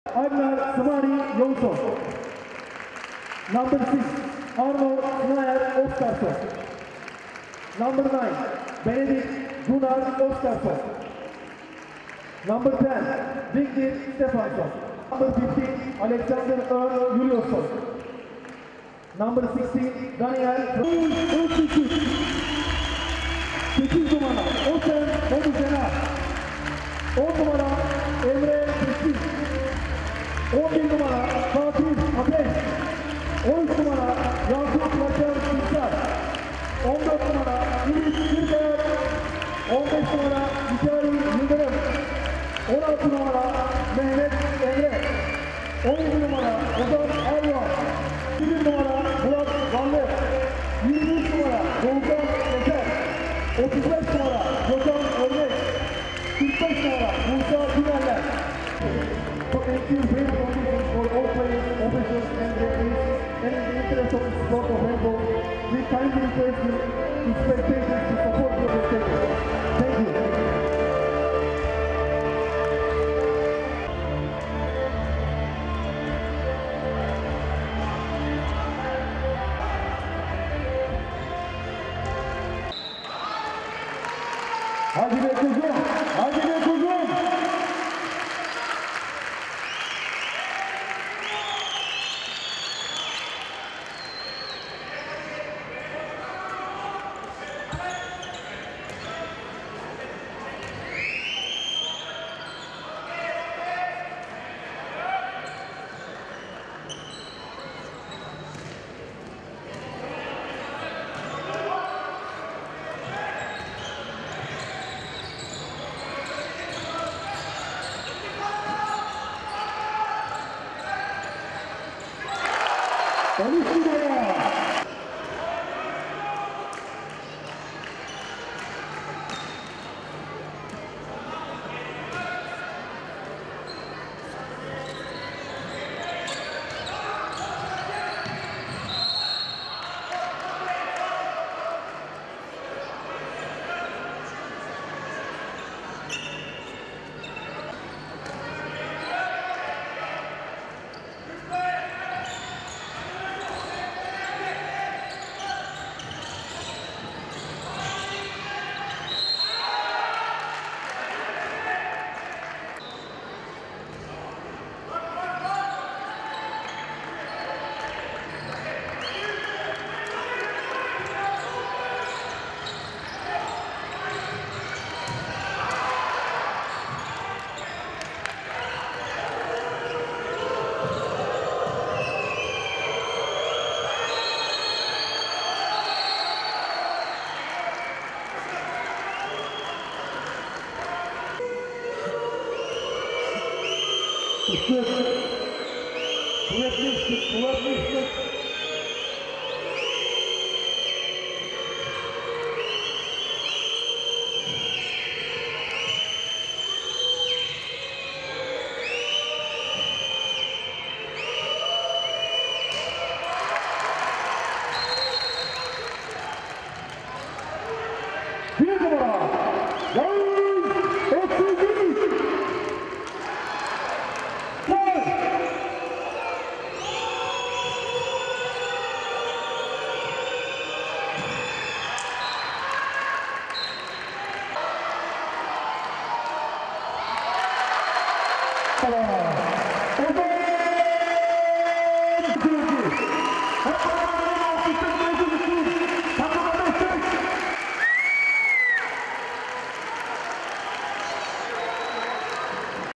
Challenge Kupası Beykoz Belediyespor 30 - Valur 31 | Hentbol
Beykoz Belediyespor Erkek Hentbol Takımı, Avrupa Challenge Kupası son 16 turu ikinci maçında Valur’a 31-30 kaybederek kupaya veda etti.
Avrupa Challenge Kupası son 16 turu ikinci maçında Beykoz Belediyespor, sahasında İzlanda temsilcisi Valur’u konuk etti. Dün oynanan ilk maçı 26-25 kaybeden Beykoz Belediyespor, ikinci maça iddialı başladı. Maçın ilk devresi 15-15’lik eşitlikle geçildi. Kıran kırana geçen ikinci yarıda ise 31-30’luk sonuçla Beykoz kupaya Son 16 Turu’nda veda eden taraf oldu.
Beykoz Belediyespor’da Yakup Simsar ve Ozan Erdoğan 6 sayıyla en skorer isim oldu. Konuk Valur’da ise en çok sayı kaydeden isimler 6’şar sayıyla Agnar Johnsson ile Vignisson oldu.

Salon: Recep Şahin Köktürk
Hakemler: Ilir Bytyqi, Syart Sakapi
Beykoz Belediyespor: Yağız, Boudjenah 5, Emre 1, Fatih, Yakup 6, Ümit 2, Mikail, Mehmet, Ozan 6, Yurii 1, Murat, Doğukan, Gökhan 5, Musa, Çağlayan 3
Başantrenör: Mesut Ilgın
Valur: Jonsson 6, Arnor Oskarsson, Benedikt Oskarsson, Stefansson 3, Valencia 1, Juliusson, Andersson, Vignisson 6, Hostert 4, Magnusson 5, Baldursson 3, Gudmundsson, Olafsson, Runarsson 3
Başantrenör: Oskar Oskarsson
İlk yarı: 15-15